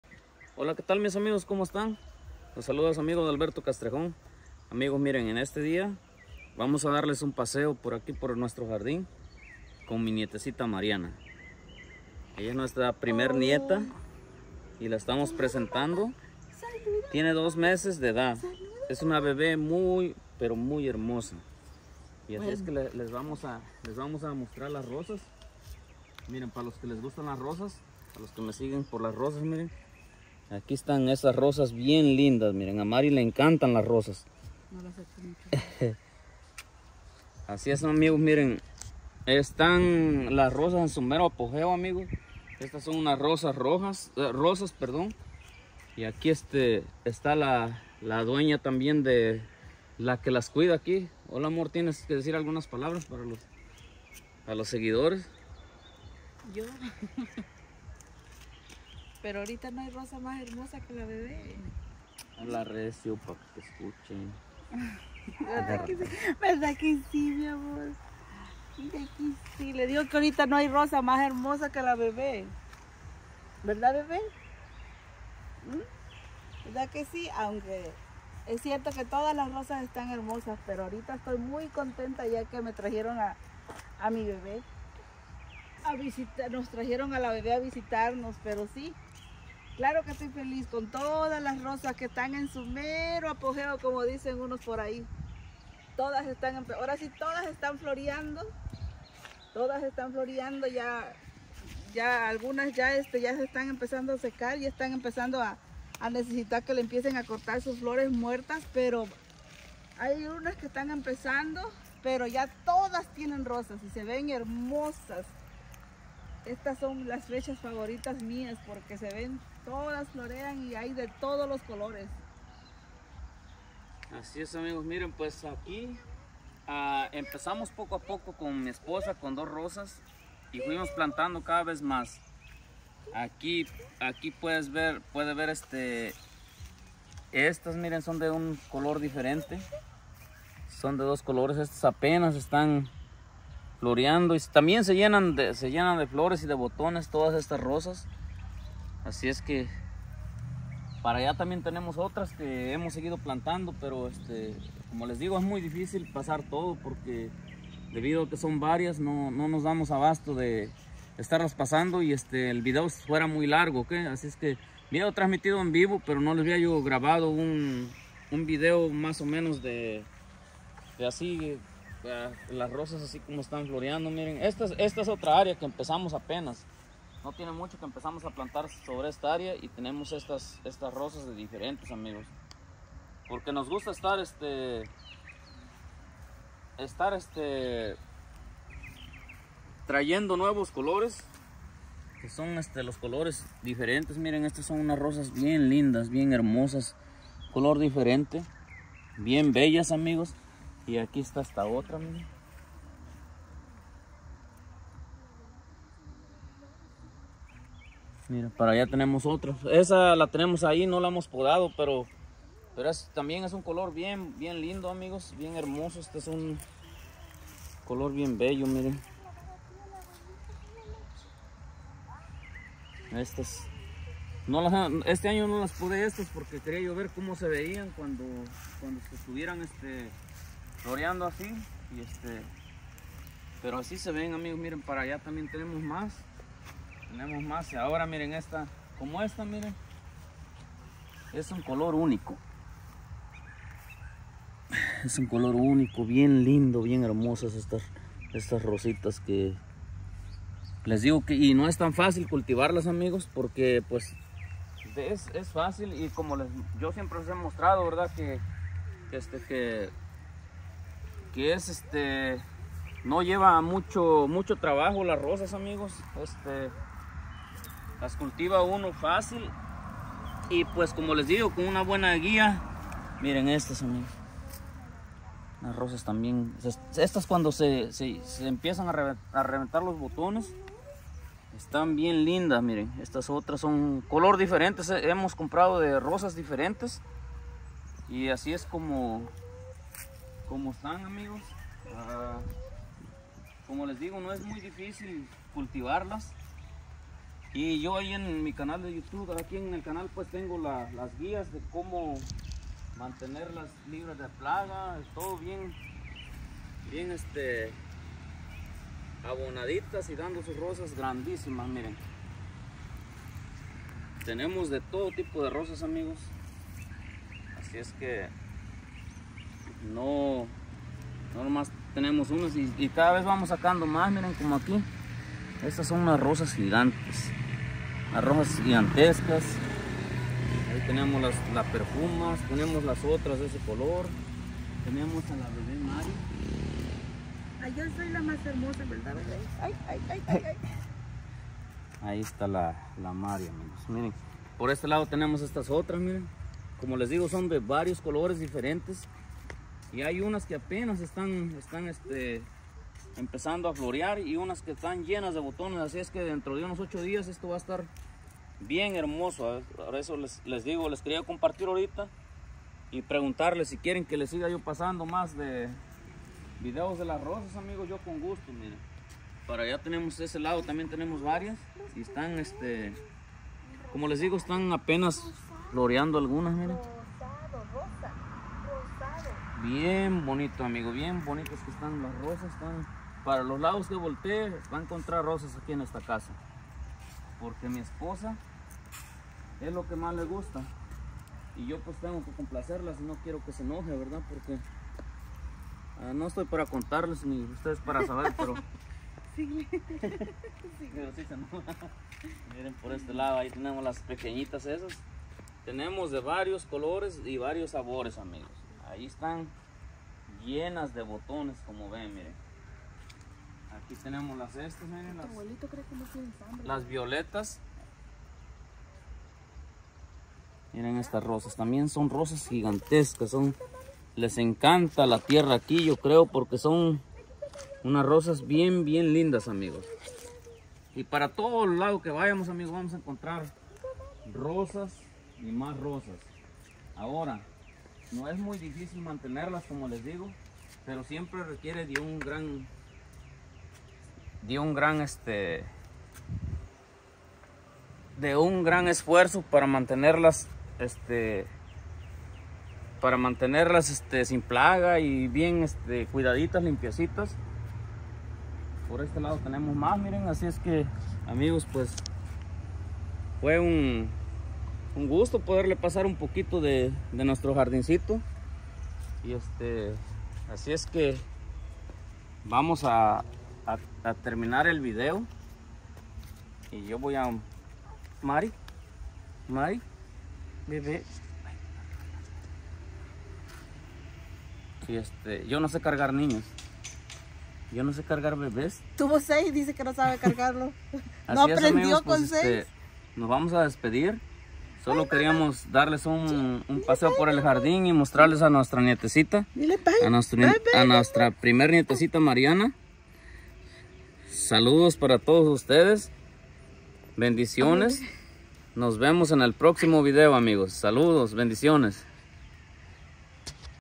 Hoy les presentamos a nuestra nieta Mariana y les damos un paseo por el jardín
En este video les presentamos a nuestra nieta y les damos un recorrido por nuestro jardín